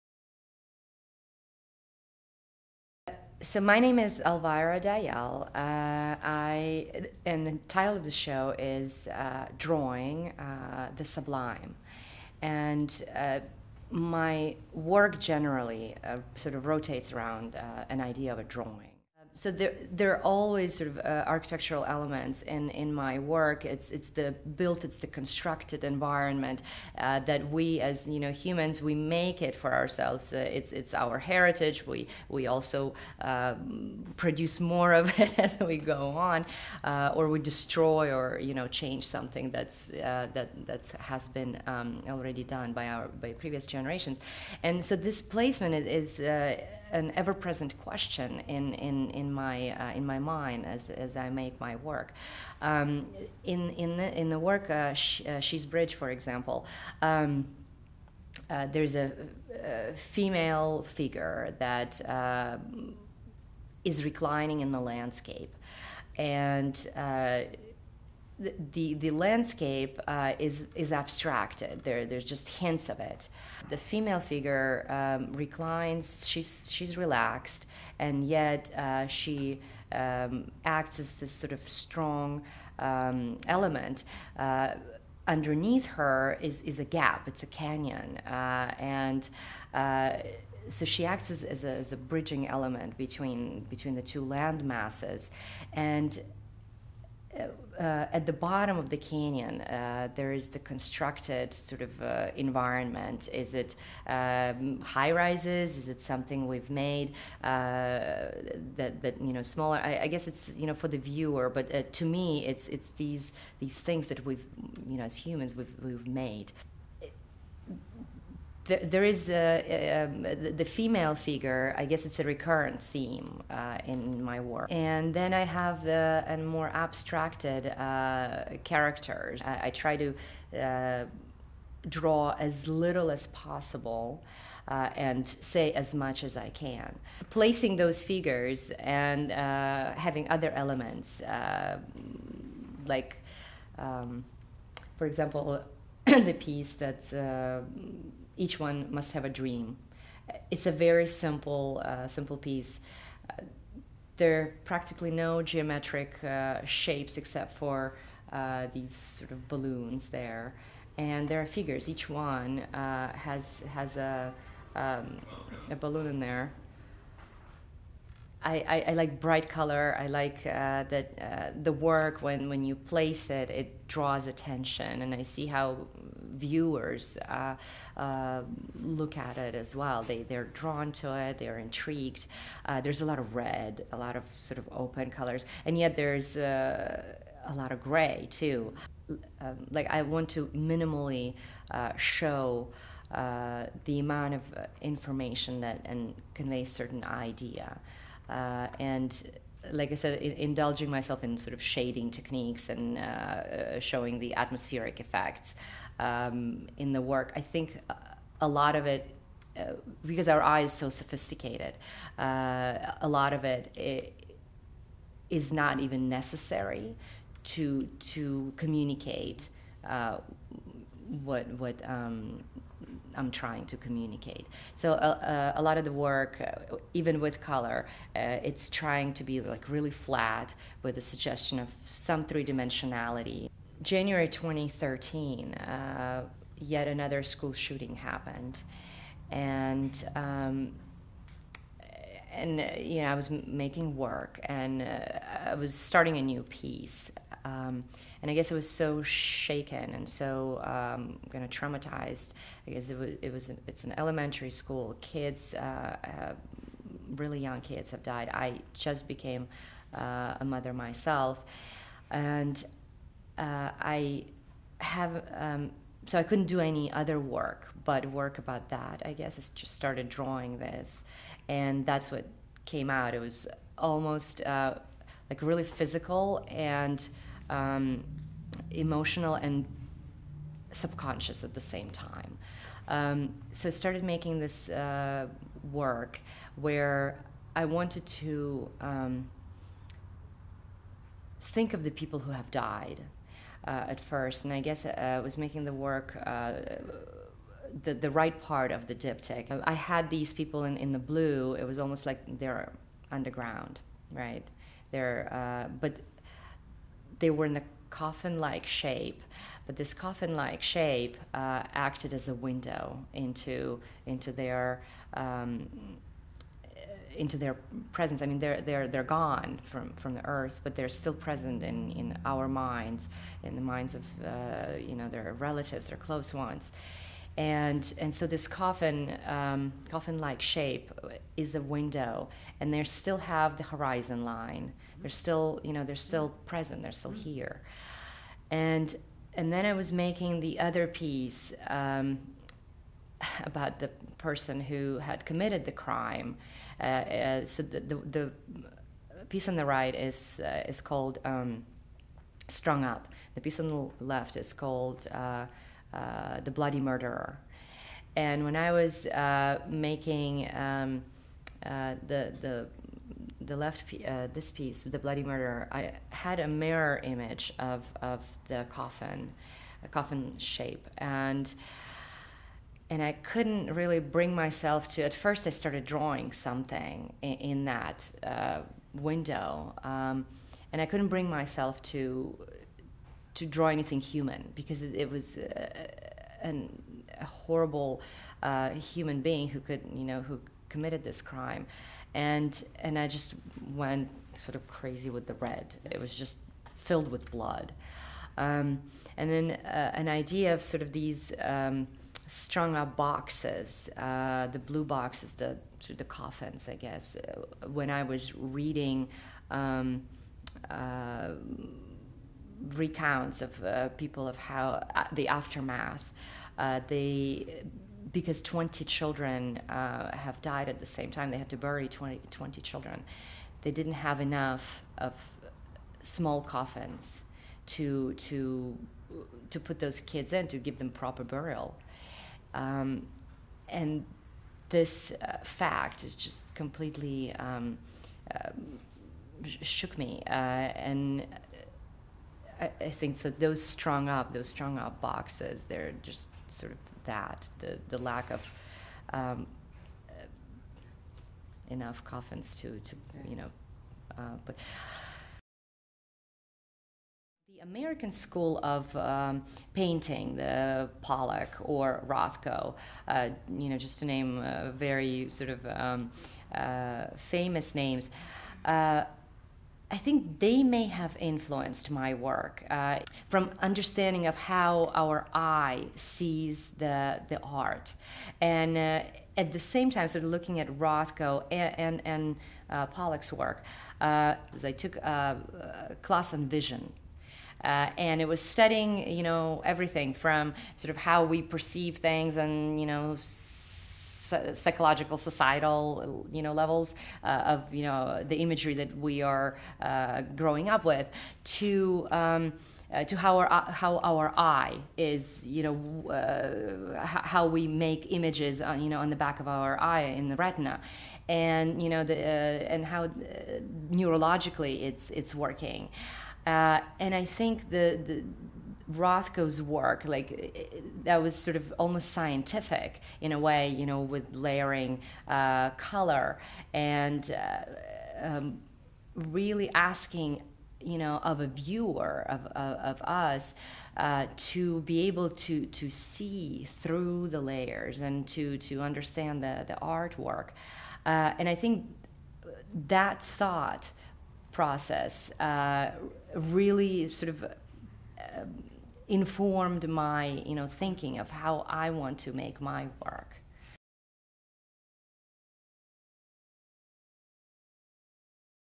Elvira Dayel Interview– Soft Pastel on Watercolor Paper
A Ukrainian native, I am currently based in San Francisco Bay Area. I work in series of large abstract works of drawings done in soft/dry pastel on paper. My work may be described as conceptually surreal influenced by modern architecture / spacial organization, design & thought of what occupied spaces should be and how they ought to function. In my work I reference the 20th century Russian avant-garde, the Constructivism and Suprematism, as well as such early modernists as Matisse, specifically his “cut-outs”. I’d like to think of my work as having its own original XXI century voice.
Trained as an artist & an architect, I also work in new media including digital rendering (surface & space visualization) and 3d-printing. My work is interwoven and interconnected between and within the media I use. Using strong, carefully considered color and bold geometric form, I frequently incorporate depictions of the female archetype. The female figure is often placed alone in an invented landscape, color and line announce her presence. Her voice, her presence, are loud, vivid, red.
I see myself as a minimalist using only as much as needed to convey an idea. The color I use carries its own vision. Invented landscapes are new realities, they are constructs. They provide a vision for an abstracted notion of a place and space. In my work the landscape is deconstructed and re-imagined as a new notion. My work is my metaphor for searching, and my landscapes & invented places implicate the presence of the unknown. Creating sculpture begins in the same way as my drawings, as a conceptual idea evolves, I begin the process of the sculpture either in a 3D program or by hand sketching. There is a deeper relationship between the works of pastel on paper and sculpture that is intriguing to me. Both are rooted in the same thinking and begin as powder: the powder compressed as chalk for the works on paper, and the powder in the 3D printing process assembled into a new material and shape.
My process in creating work may be described as a kind of blindness at first. As I am working through the drawing there is a moment when all of a sudden there is a slit, a cranny of light which opens up – and then I can see! At this moment the doors of a new vision, a new perception open up. As the fog of the reality shifts and morphs, the fuzzy outlines take on a different meaning, and ideas and concepts stand out. This opening of the subconscious enables me to create new work manifesting my personal observation of the world. In the fog I search for the sublime – for me it is when the dark & grey become poetry.